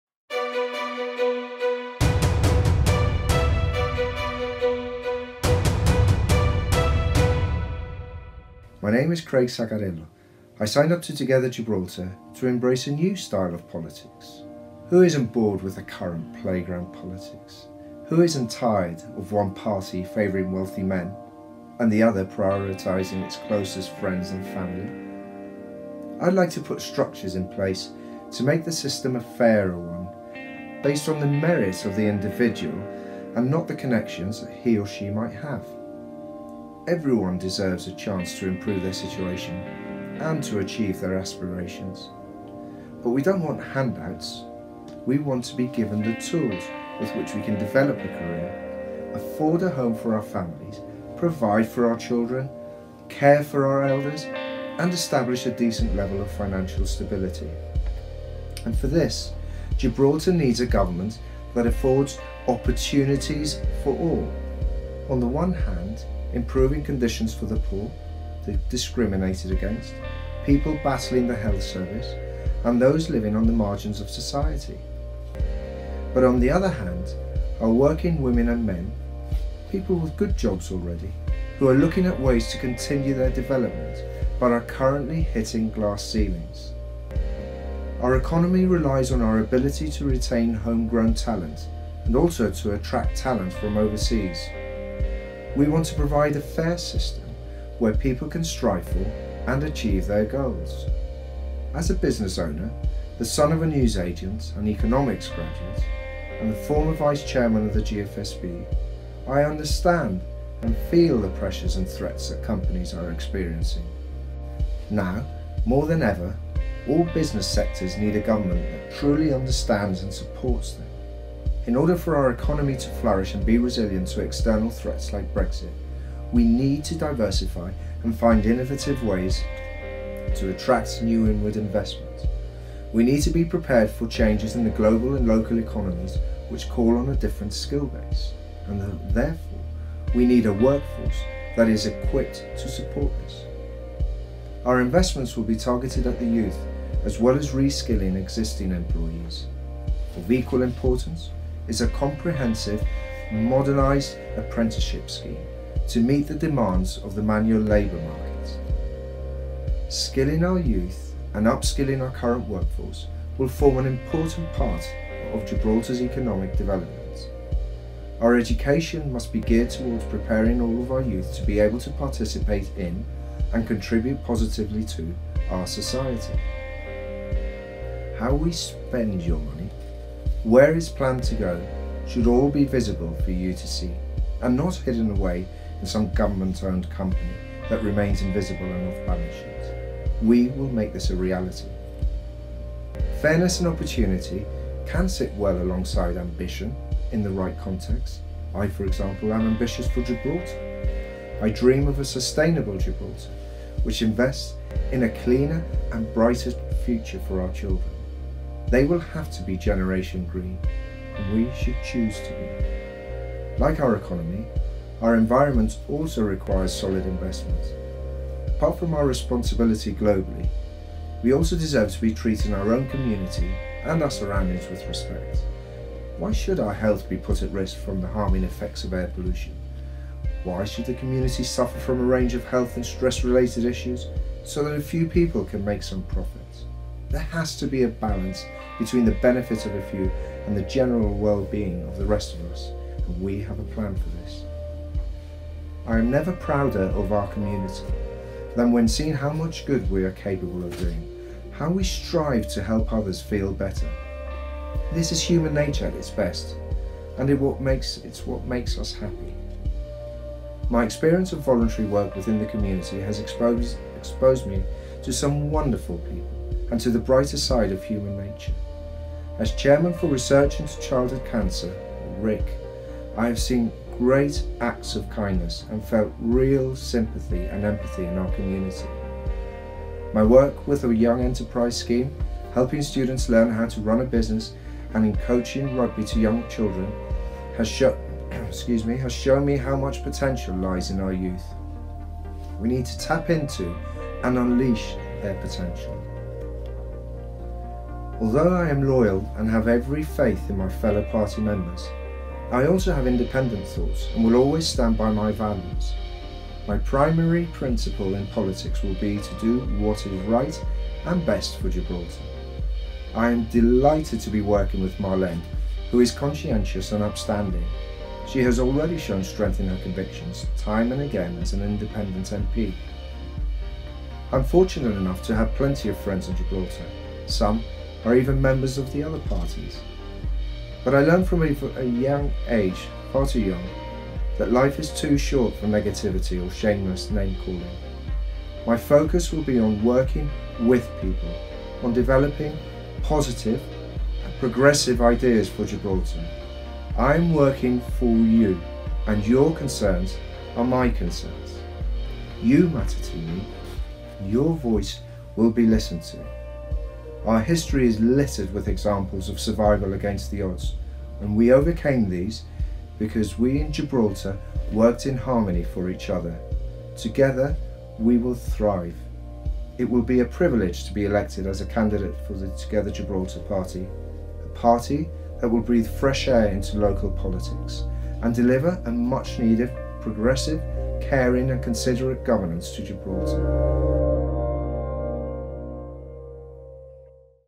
Craig Sacarello
Together Gibraltar candidate video for Craig Sacarello